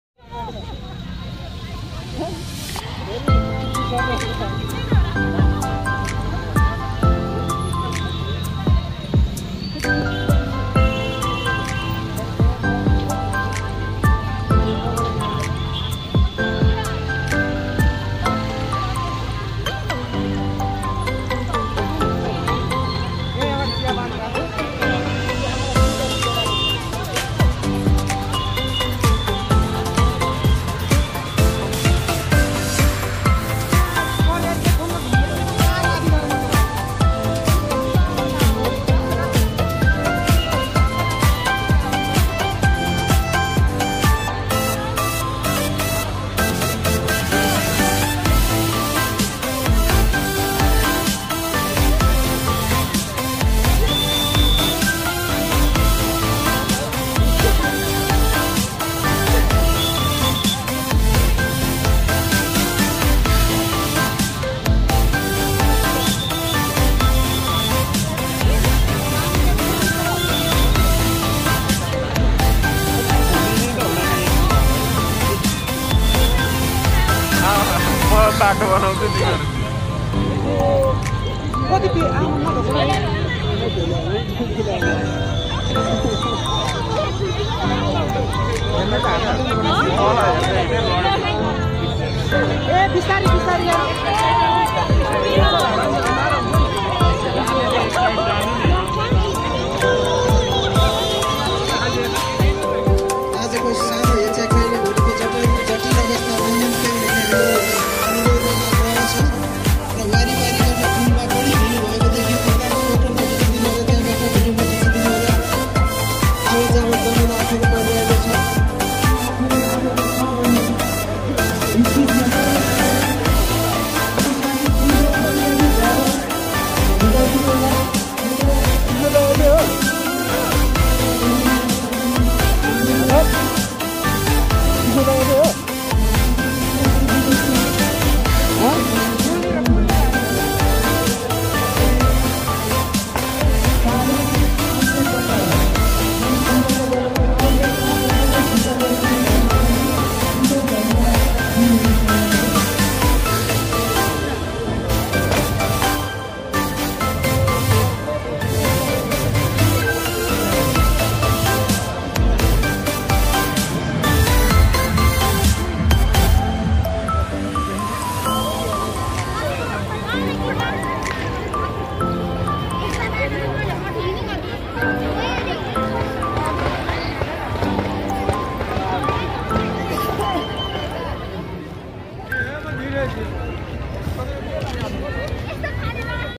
Chhath Puja
Pasupathi area ma chhath puja garidai